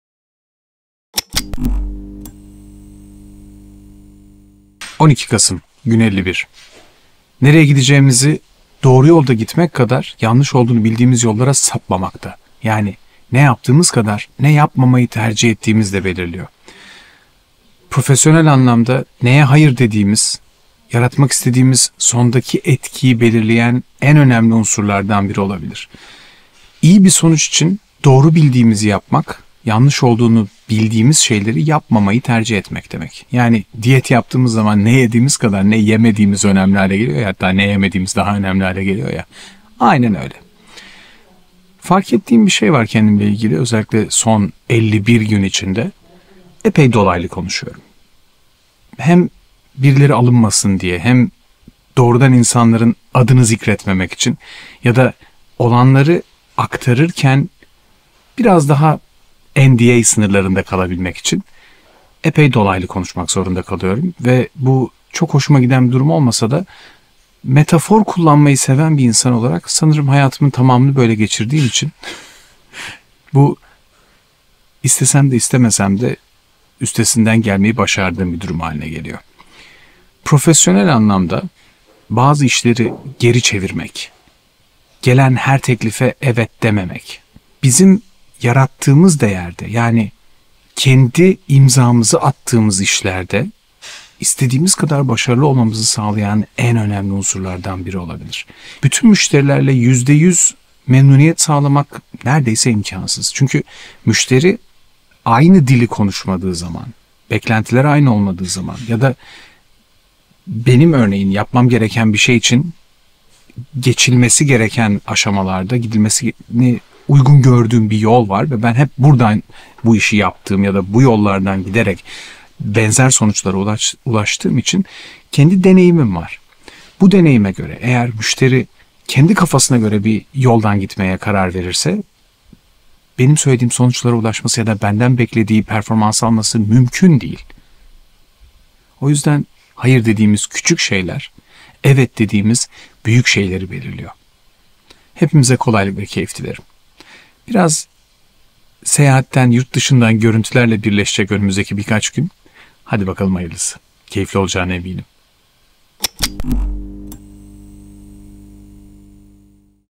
iyi | doğru | zinde podshow: GüN051 HAYIRSEVERLİK
23.09.23 itibariyle başlayan bir projede 51. yayın

Nelere hayır diyebildiğimize bağlı olarak "Evet" dediklerimizin bir değeri olduğuna inanıyorum.

23 Eylül’den 2023 yılı sonuna kadar tam 100 gün var!
29 Ekim’de 100. yaşını kutlayacağımız Cumhuriyetimizin
101. yaşının ilk 100 günü ve sonrası boyunca her gün
iYi | DOĞRU | ZiNDE yaşamak isteyenler için

İYİ içerik İYİ ürün İYİ marka İYİ hizmet İYİ insan İYİ niyet

DOĞRU ifade DOĞRU sistem DOĞRU yöntem

ZİNDE beden ZİNDE zihin ZİNDE ruh

özel konular ve konuklarla birlikte

video podcast = podshow

etkinlikler,  canlı yayınlar

aralıksız 100 gün